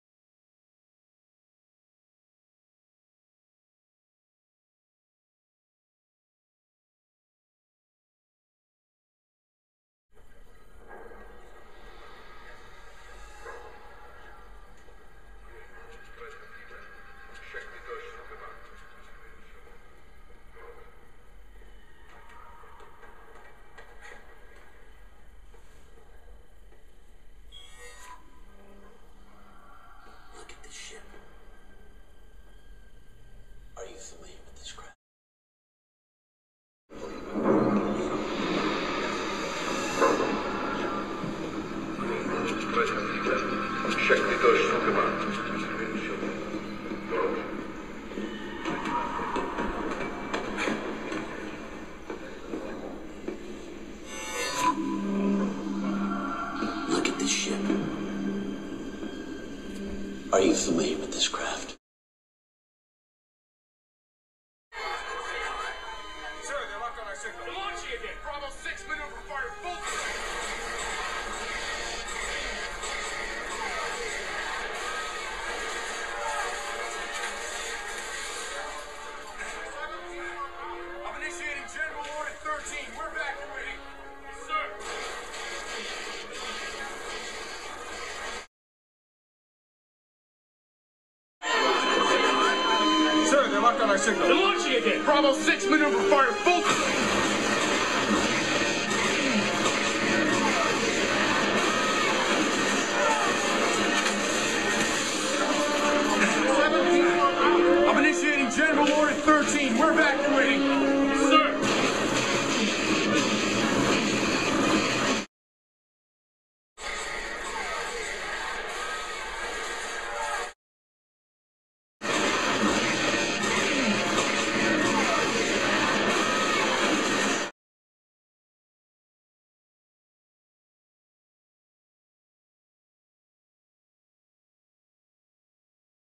Laptops Speakers ihm79 with ipad
See a movie on the ipad without and with the ihm79 speakers for laptops,
ipads, ipods, iphones, or just about any mp3 player or smart phone.